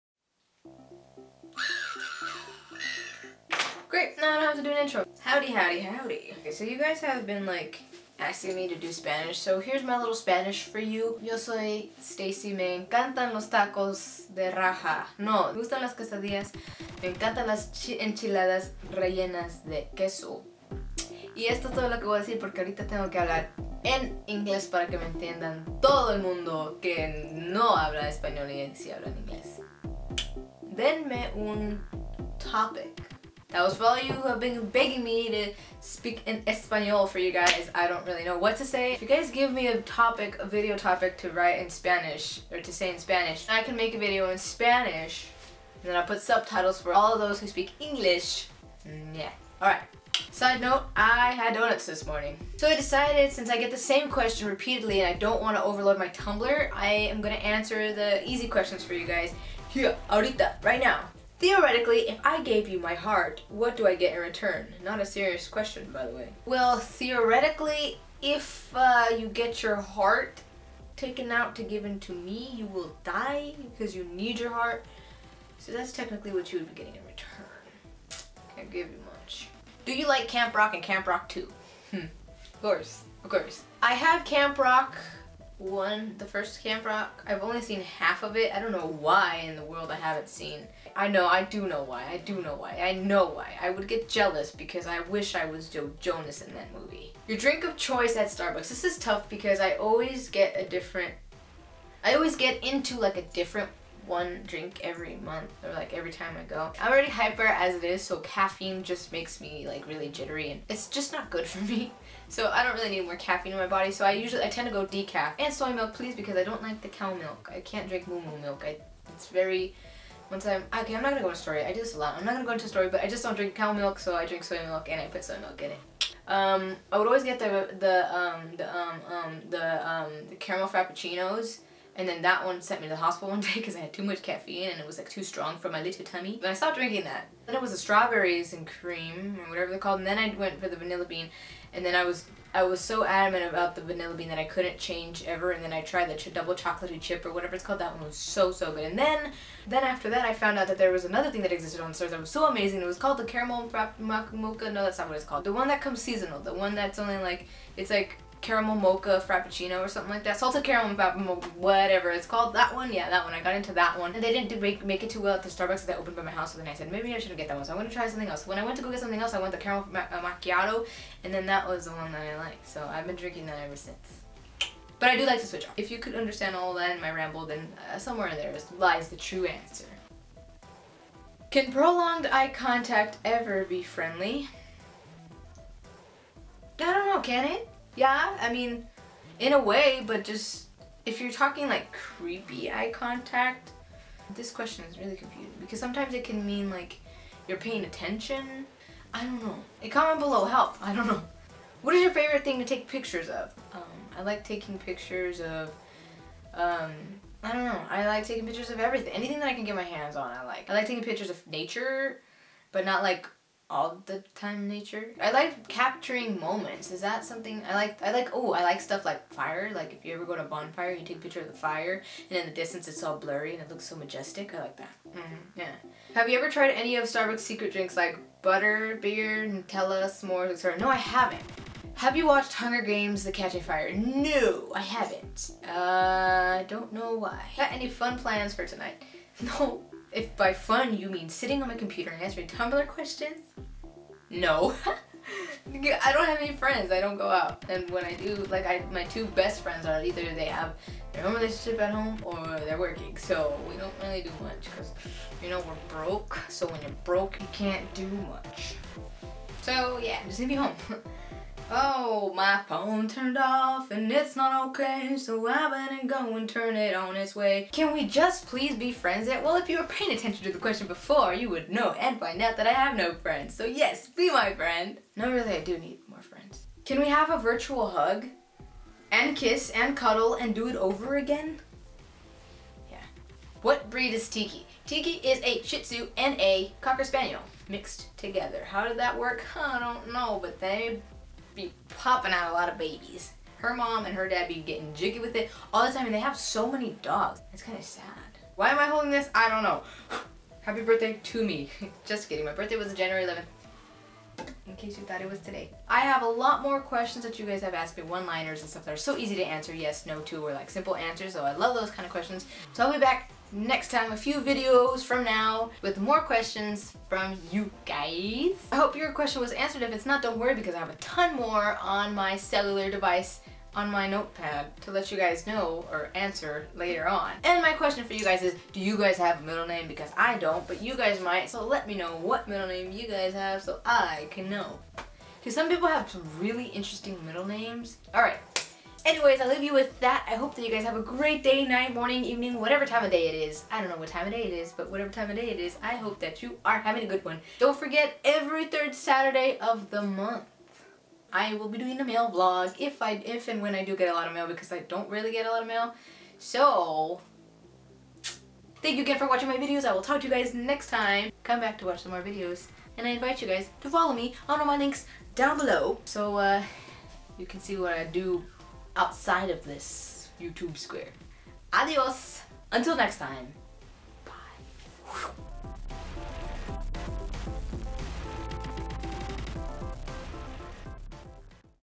If I Gave You My Heart
Here we are, a compilation of answers to some of the common questions I get on tumblr. It was fun, how do you like my mustache? Thank you Jessica R form KY for the mustache keychain, I LOVE it!